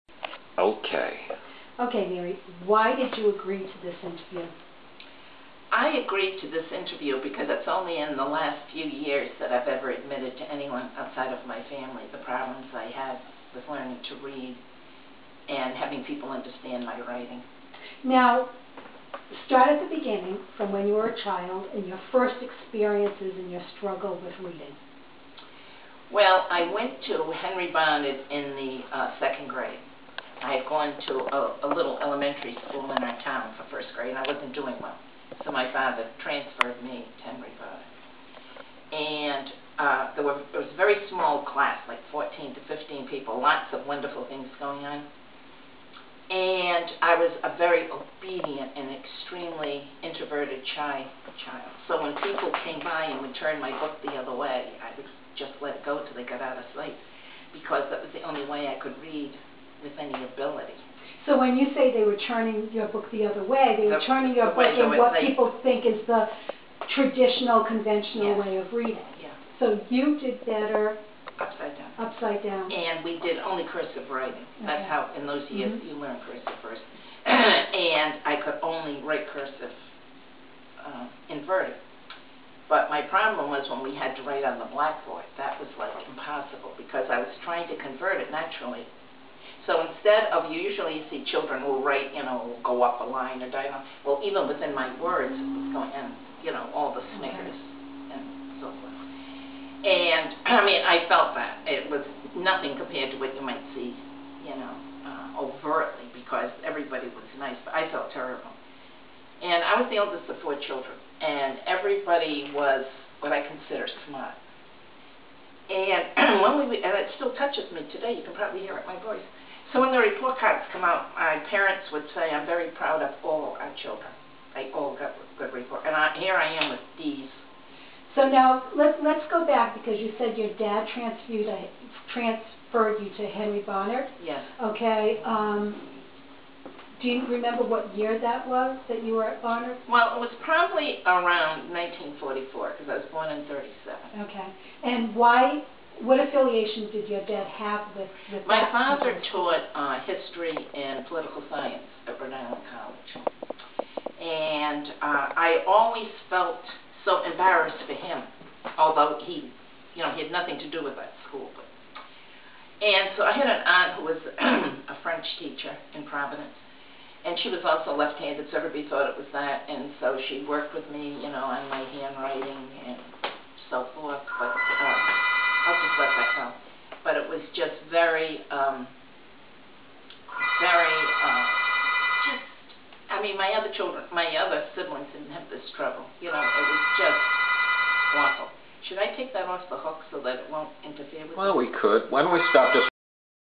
74 Year Old Reading Teacher Was PI Reader and Writer as a Child (Interview Pt.1)
When Mary Frappier heard that MIT was conducting a study on Upside-Down Reading in a local school system, she contacted my wife and I with a fascinating story. Here's part 1 of the interview. For more information about Print Inverted Reading and Writing please go